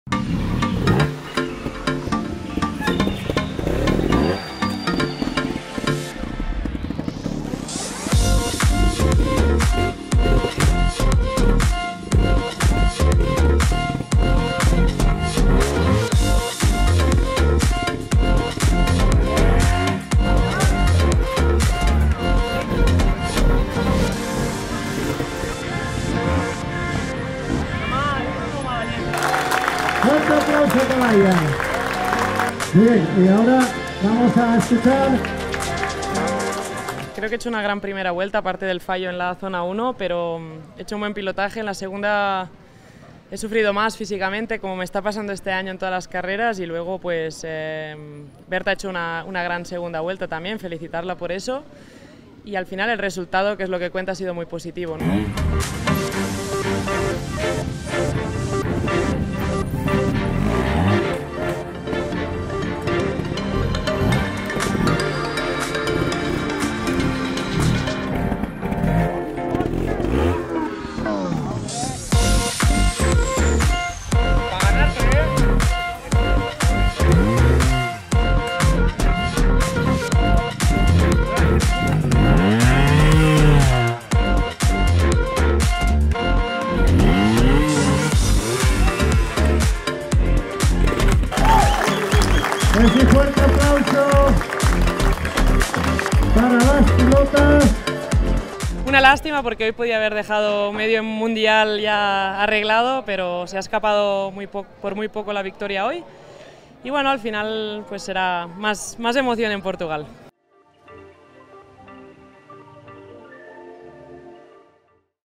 Highlights Laia Sanz TrialGP Spain, Pobladura de las Regueras 2021
Los mejores momentos y las declaraciones de Laia Sanz en el TrialGP de España celebrado en Pobladura de las Regueras.

🇬🇧 Laia Sanz's highlights and statements at the TrialGP of Spain held in Pobladura de las Regueras.